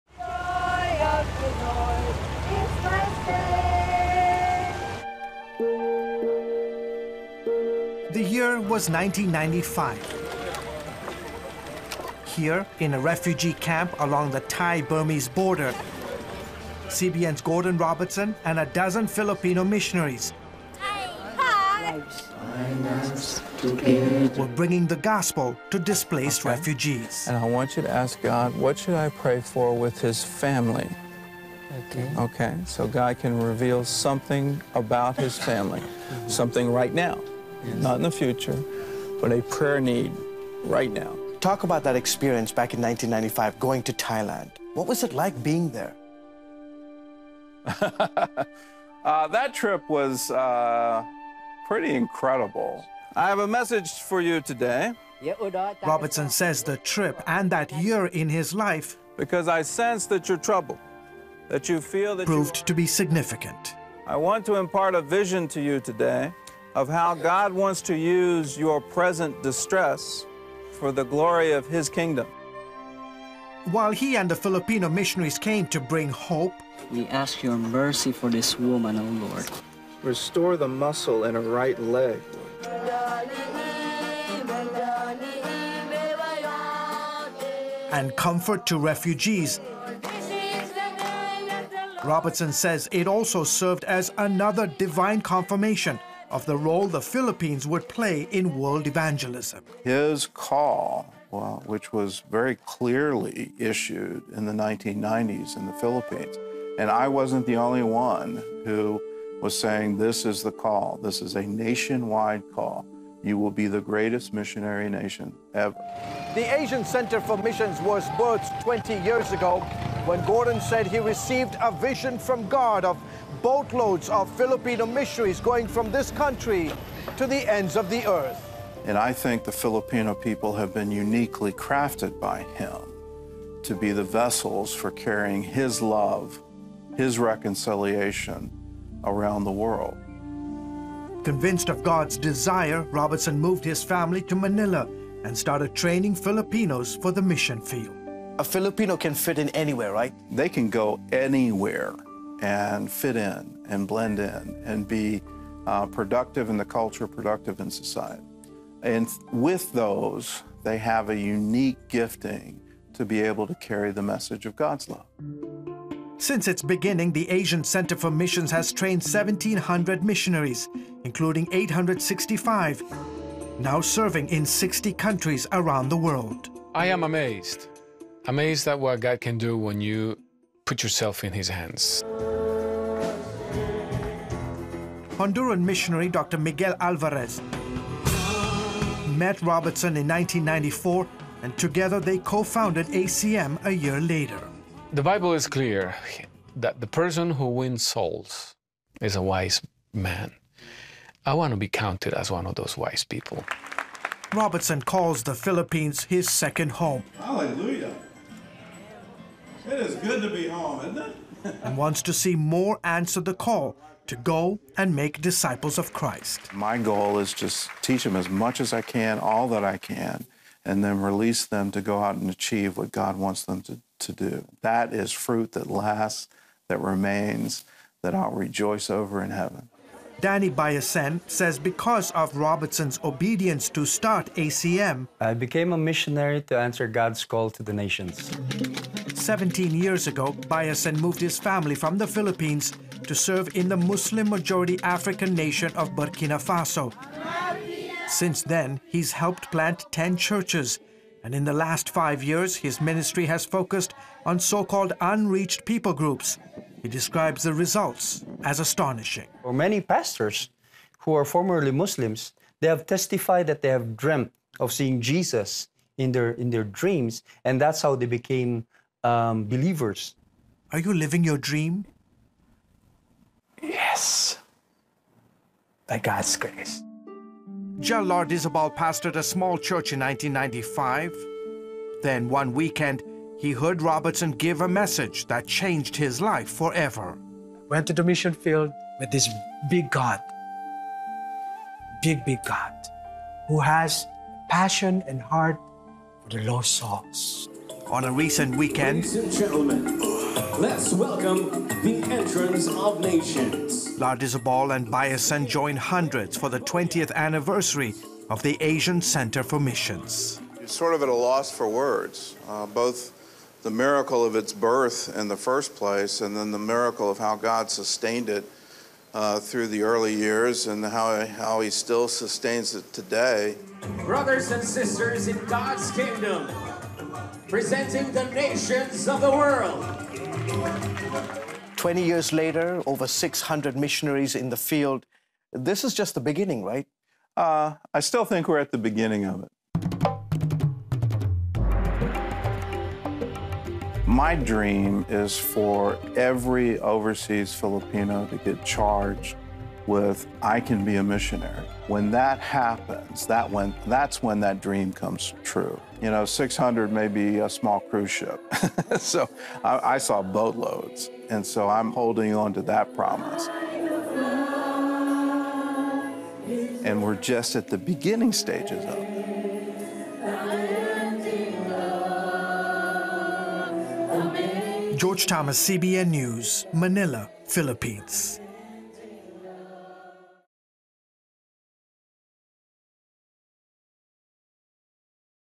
'I Saw Boatloads' of Filipinos Going as Missionaries
It's been 20 years since Gordon Robertson started a Christian ministry in the Philippines, and now Filipino Christians are spreading the gospel to the world.